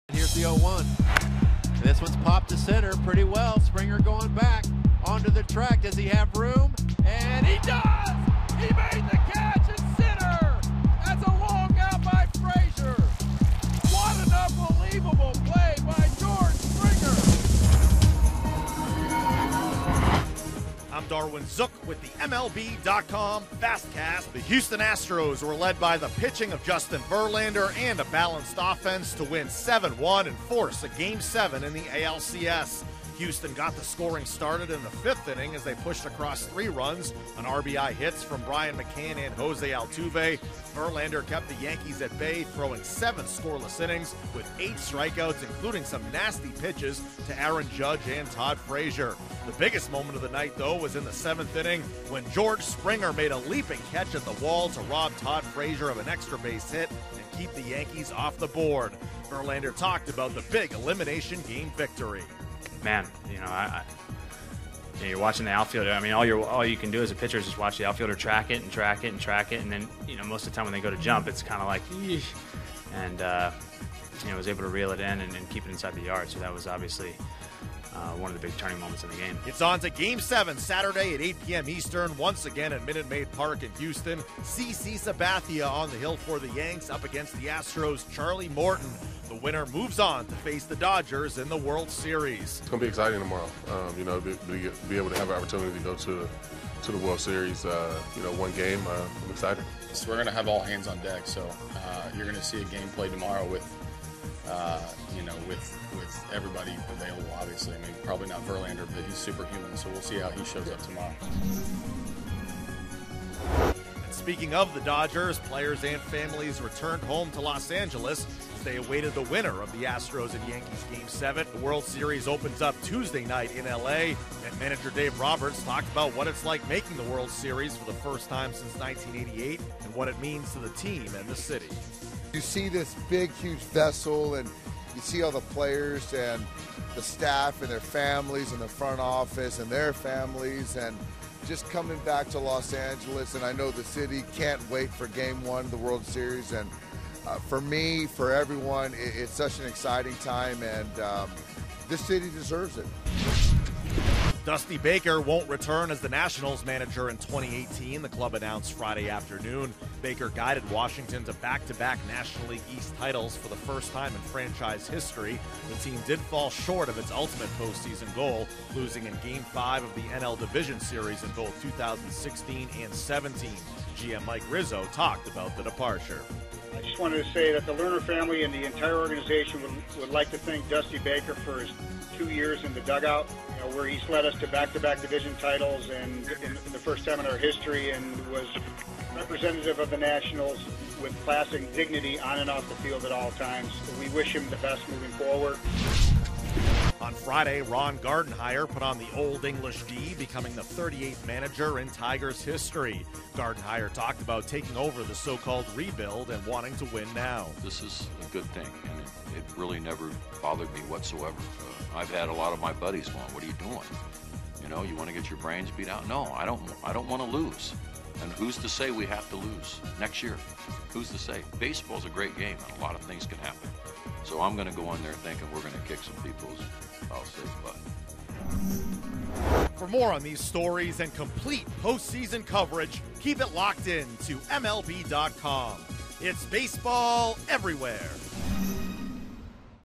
MLB.com FastCast: Astros win Game 6 - 10/20/17
Aided by Jose Altuve's bat and George Springer's glove, Justin Verlander stifles the Yanks to avoid elimination and force Game 7 of the ALCS